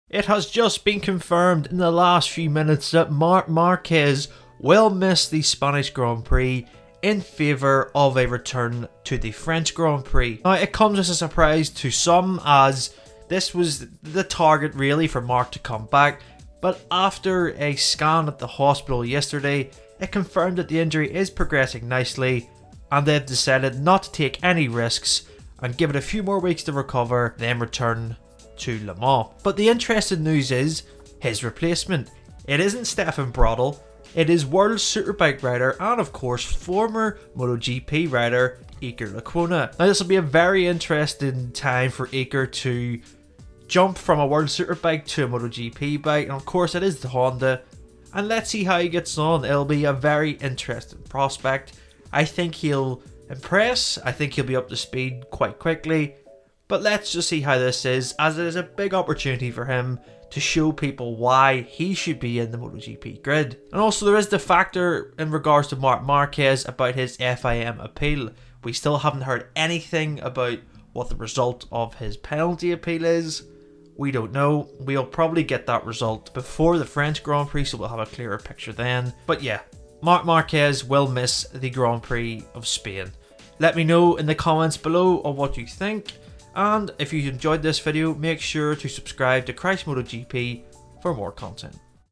Marc Marquez will MISS the Spanish GP ❌
Marc Marquez will not “take any risks” and will miss this weekend’s Spanish MotoGP in Jerez.

The Repsol Honda will miss the fourth round of the 2023 season as he continues recovering from the hand injury he sustained when he crashed into Miguel Oliveira in Portimao.

Iker Lecuona, the Honda WorldSBK rider, will replace Marquez for the Spanish MotoGP. Marquez will turn his attention to returning for the French MotoGP in Le Mans on May 14.

Marquez underwent successful surgery to repair a broken hand after the first weekend of the season and, ahead of Jerez, Honda confirmed: “Due to the nature of the fracture and the time elapsed, the medical team led by Dr. Roger de Oña together with Marquez, have decided to continue with his recovery.”

Marquez said: “[Tuesday] we did another CT scan and it has been confirmed that, despite the fact that the injury is progressing favourably, the bone has not yet finished healing and racing in Jerez was risky."

There is still no final decision on the double long lap penalty that Marquez was hit with, after crashing into Oliveira.

The ruling was reworded when Marquez missed rounds due to injury to state that he must serve the penalty whenever he comes back, but Honda appealed this change.

The MotoGP Court of Appeal has yet to decide whether Marquez must serve the penalty at the next race he competes in.